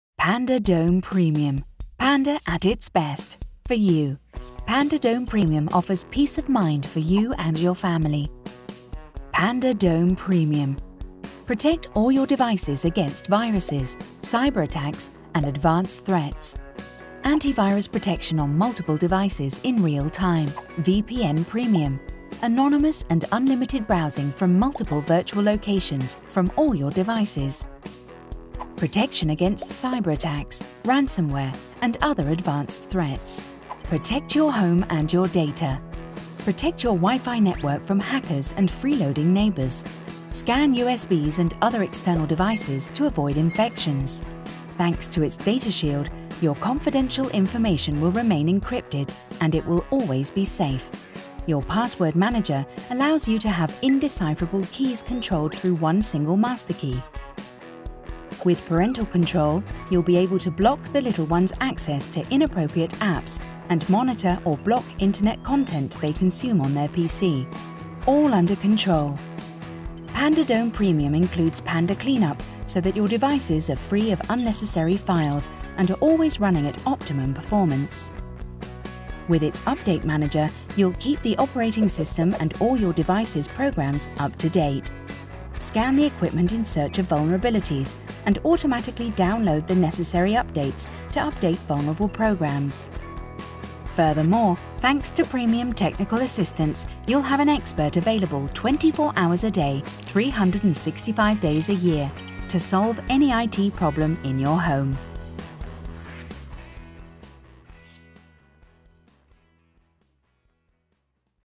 Panda Dome Premium - Panda's best for you
The best antivirus protection. PANDA DOME PREMIUM brings together Panda's best protection, privacy and performance features into a single solution. This high-end, cross-platform antivirus product allows you to enjoy all your devices with complete peace of mind.

Includes:

-Premium VPN

-Premium support

-Parental controls

-Cell phone locator

-Password manager

-Anti-ransomware protection

-Tuneup